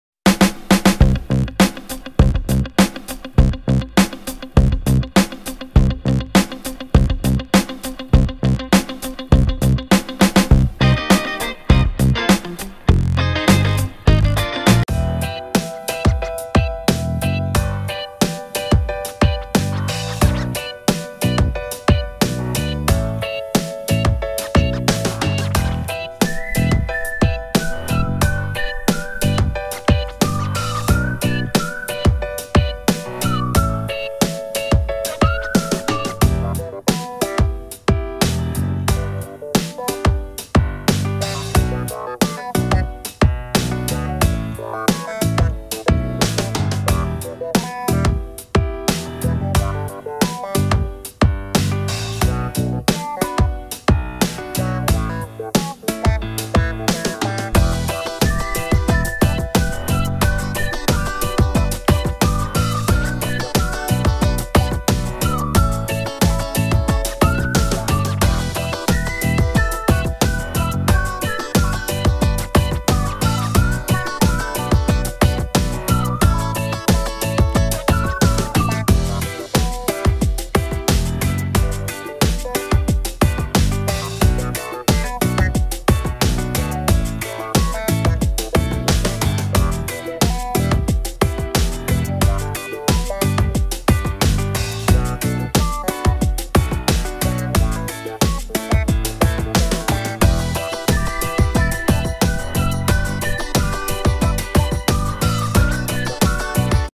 Rabo de Cavalo Despojado (verão 13/14)
Acessório usado para dar volume no cabelo é o BUMPITS e vocês encontram na use meu cupom vanessaw e ganhe desconto na hora da compra!